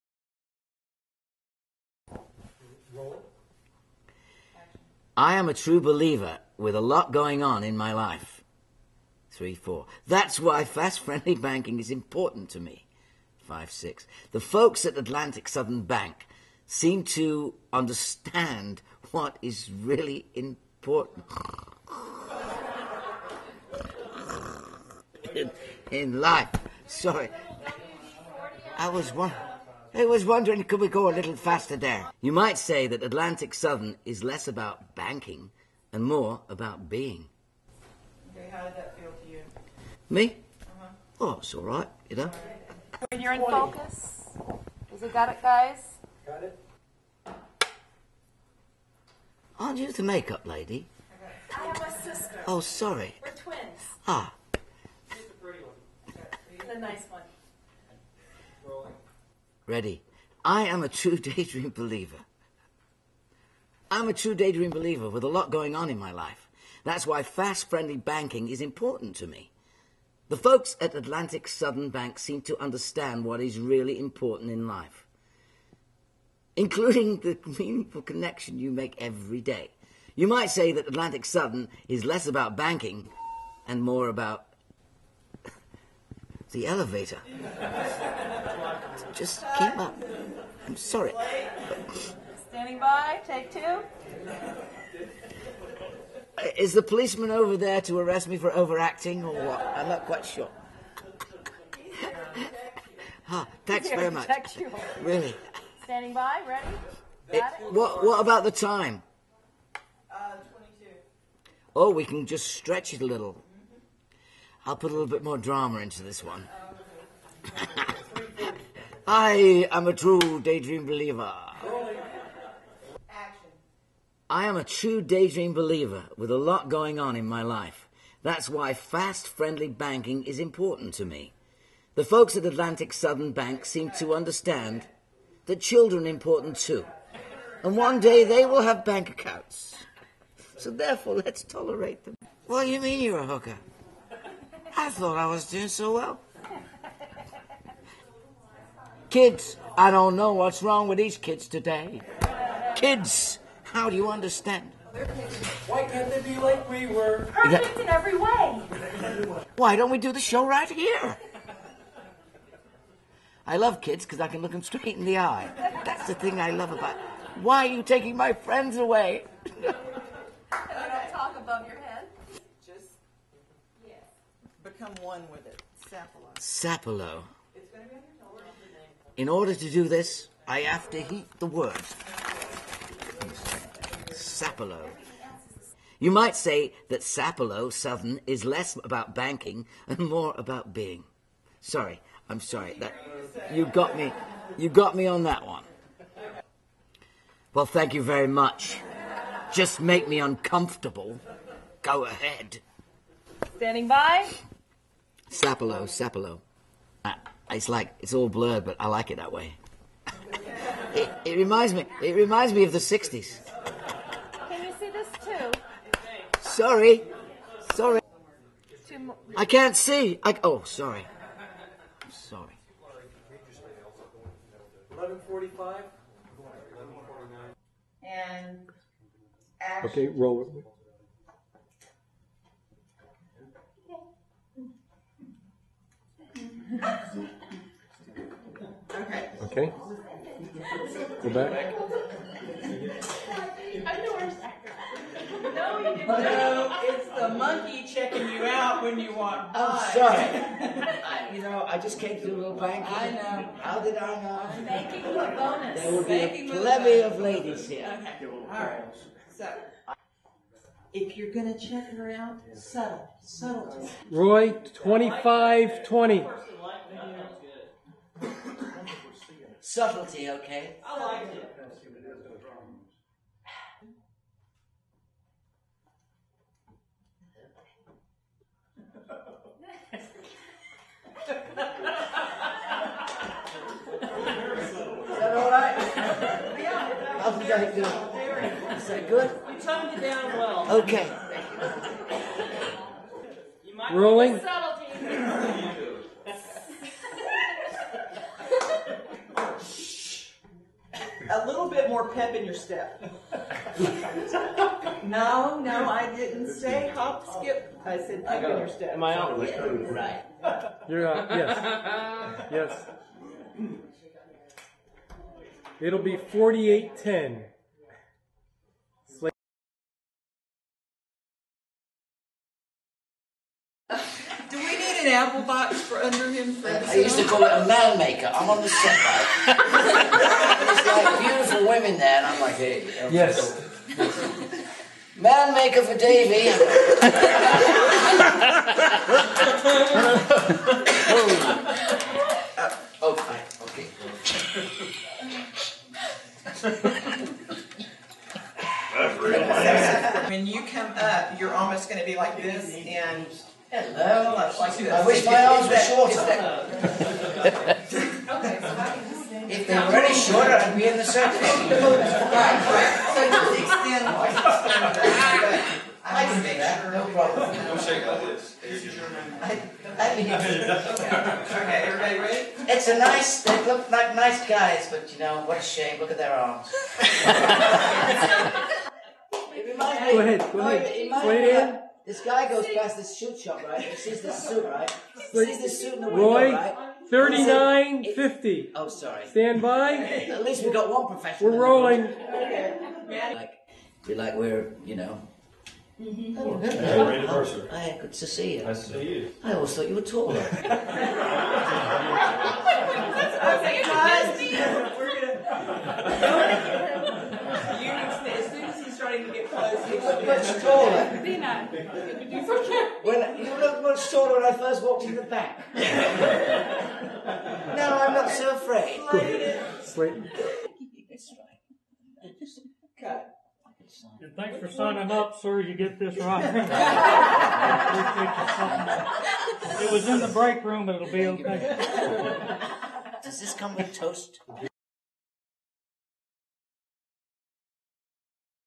Davy Jones - Outtakes from commercial shoot
This was from a bank commercial that we produced in 2008. We were shooting in a working bank, hence the elevator and people all around stopping to watch. The shoot ran over a couple of hours due to Davy being a total clown and keeping us all in stitches all day. I felt like I'd done a thousand sit-ups - it was non-stop.